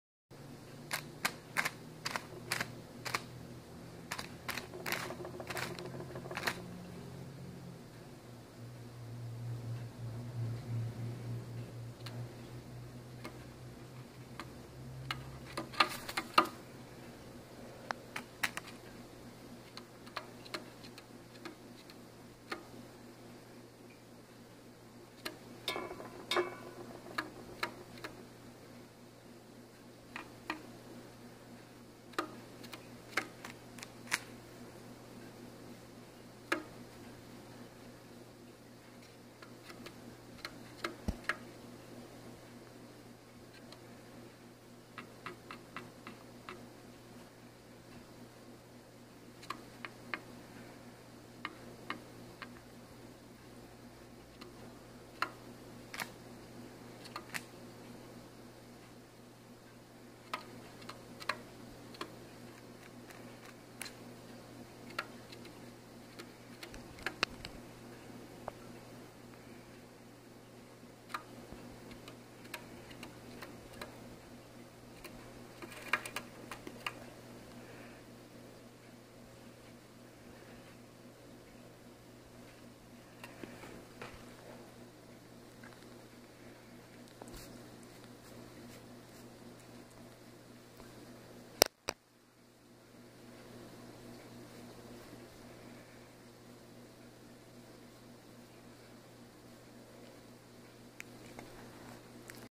Curious Pigeon
Fancy Pigeon, Spigot, walks around pecking at things.

Update: I am heartbroken to report that Spigot went into surgery on Friday August 1, 2016 around noon to remove a badly infected uterus, and after more than an hour under the anesthesia, he was not able to recover. He brought me two years of the strongest and most important love I have ever known, and his departure was peaceful. I sincerely hope that this video and others of my special little imprint bring you joy. Each one is a snapshot of our short and unfairly ephemeral time together, and a memory that I hold near and dear to my heart.